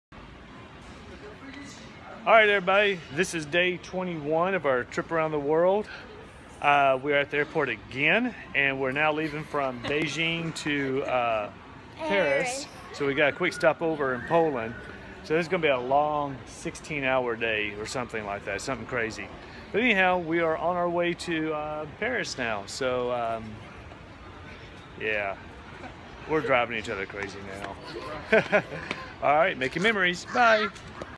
Trip Around the Word - Day 21
Leaving Beijing and going to Paris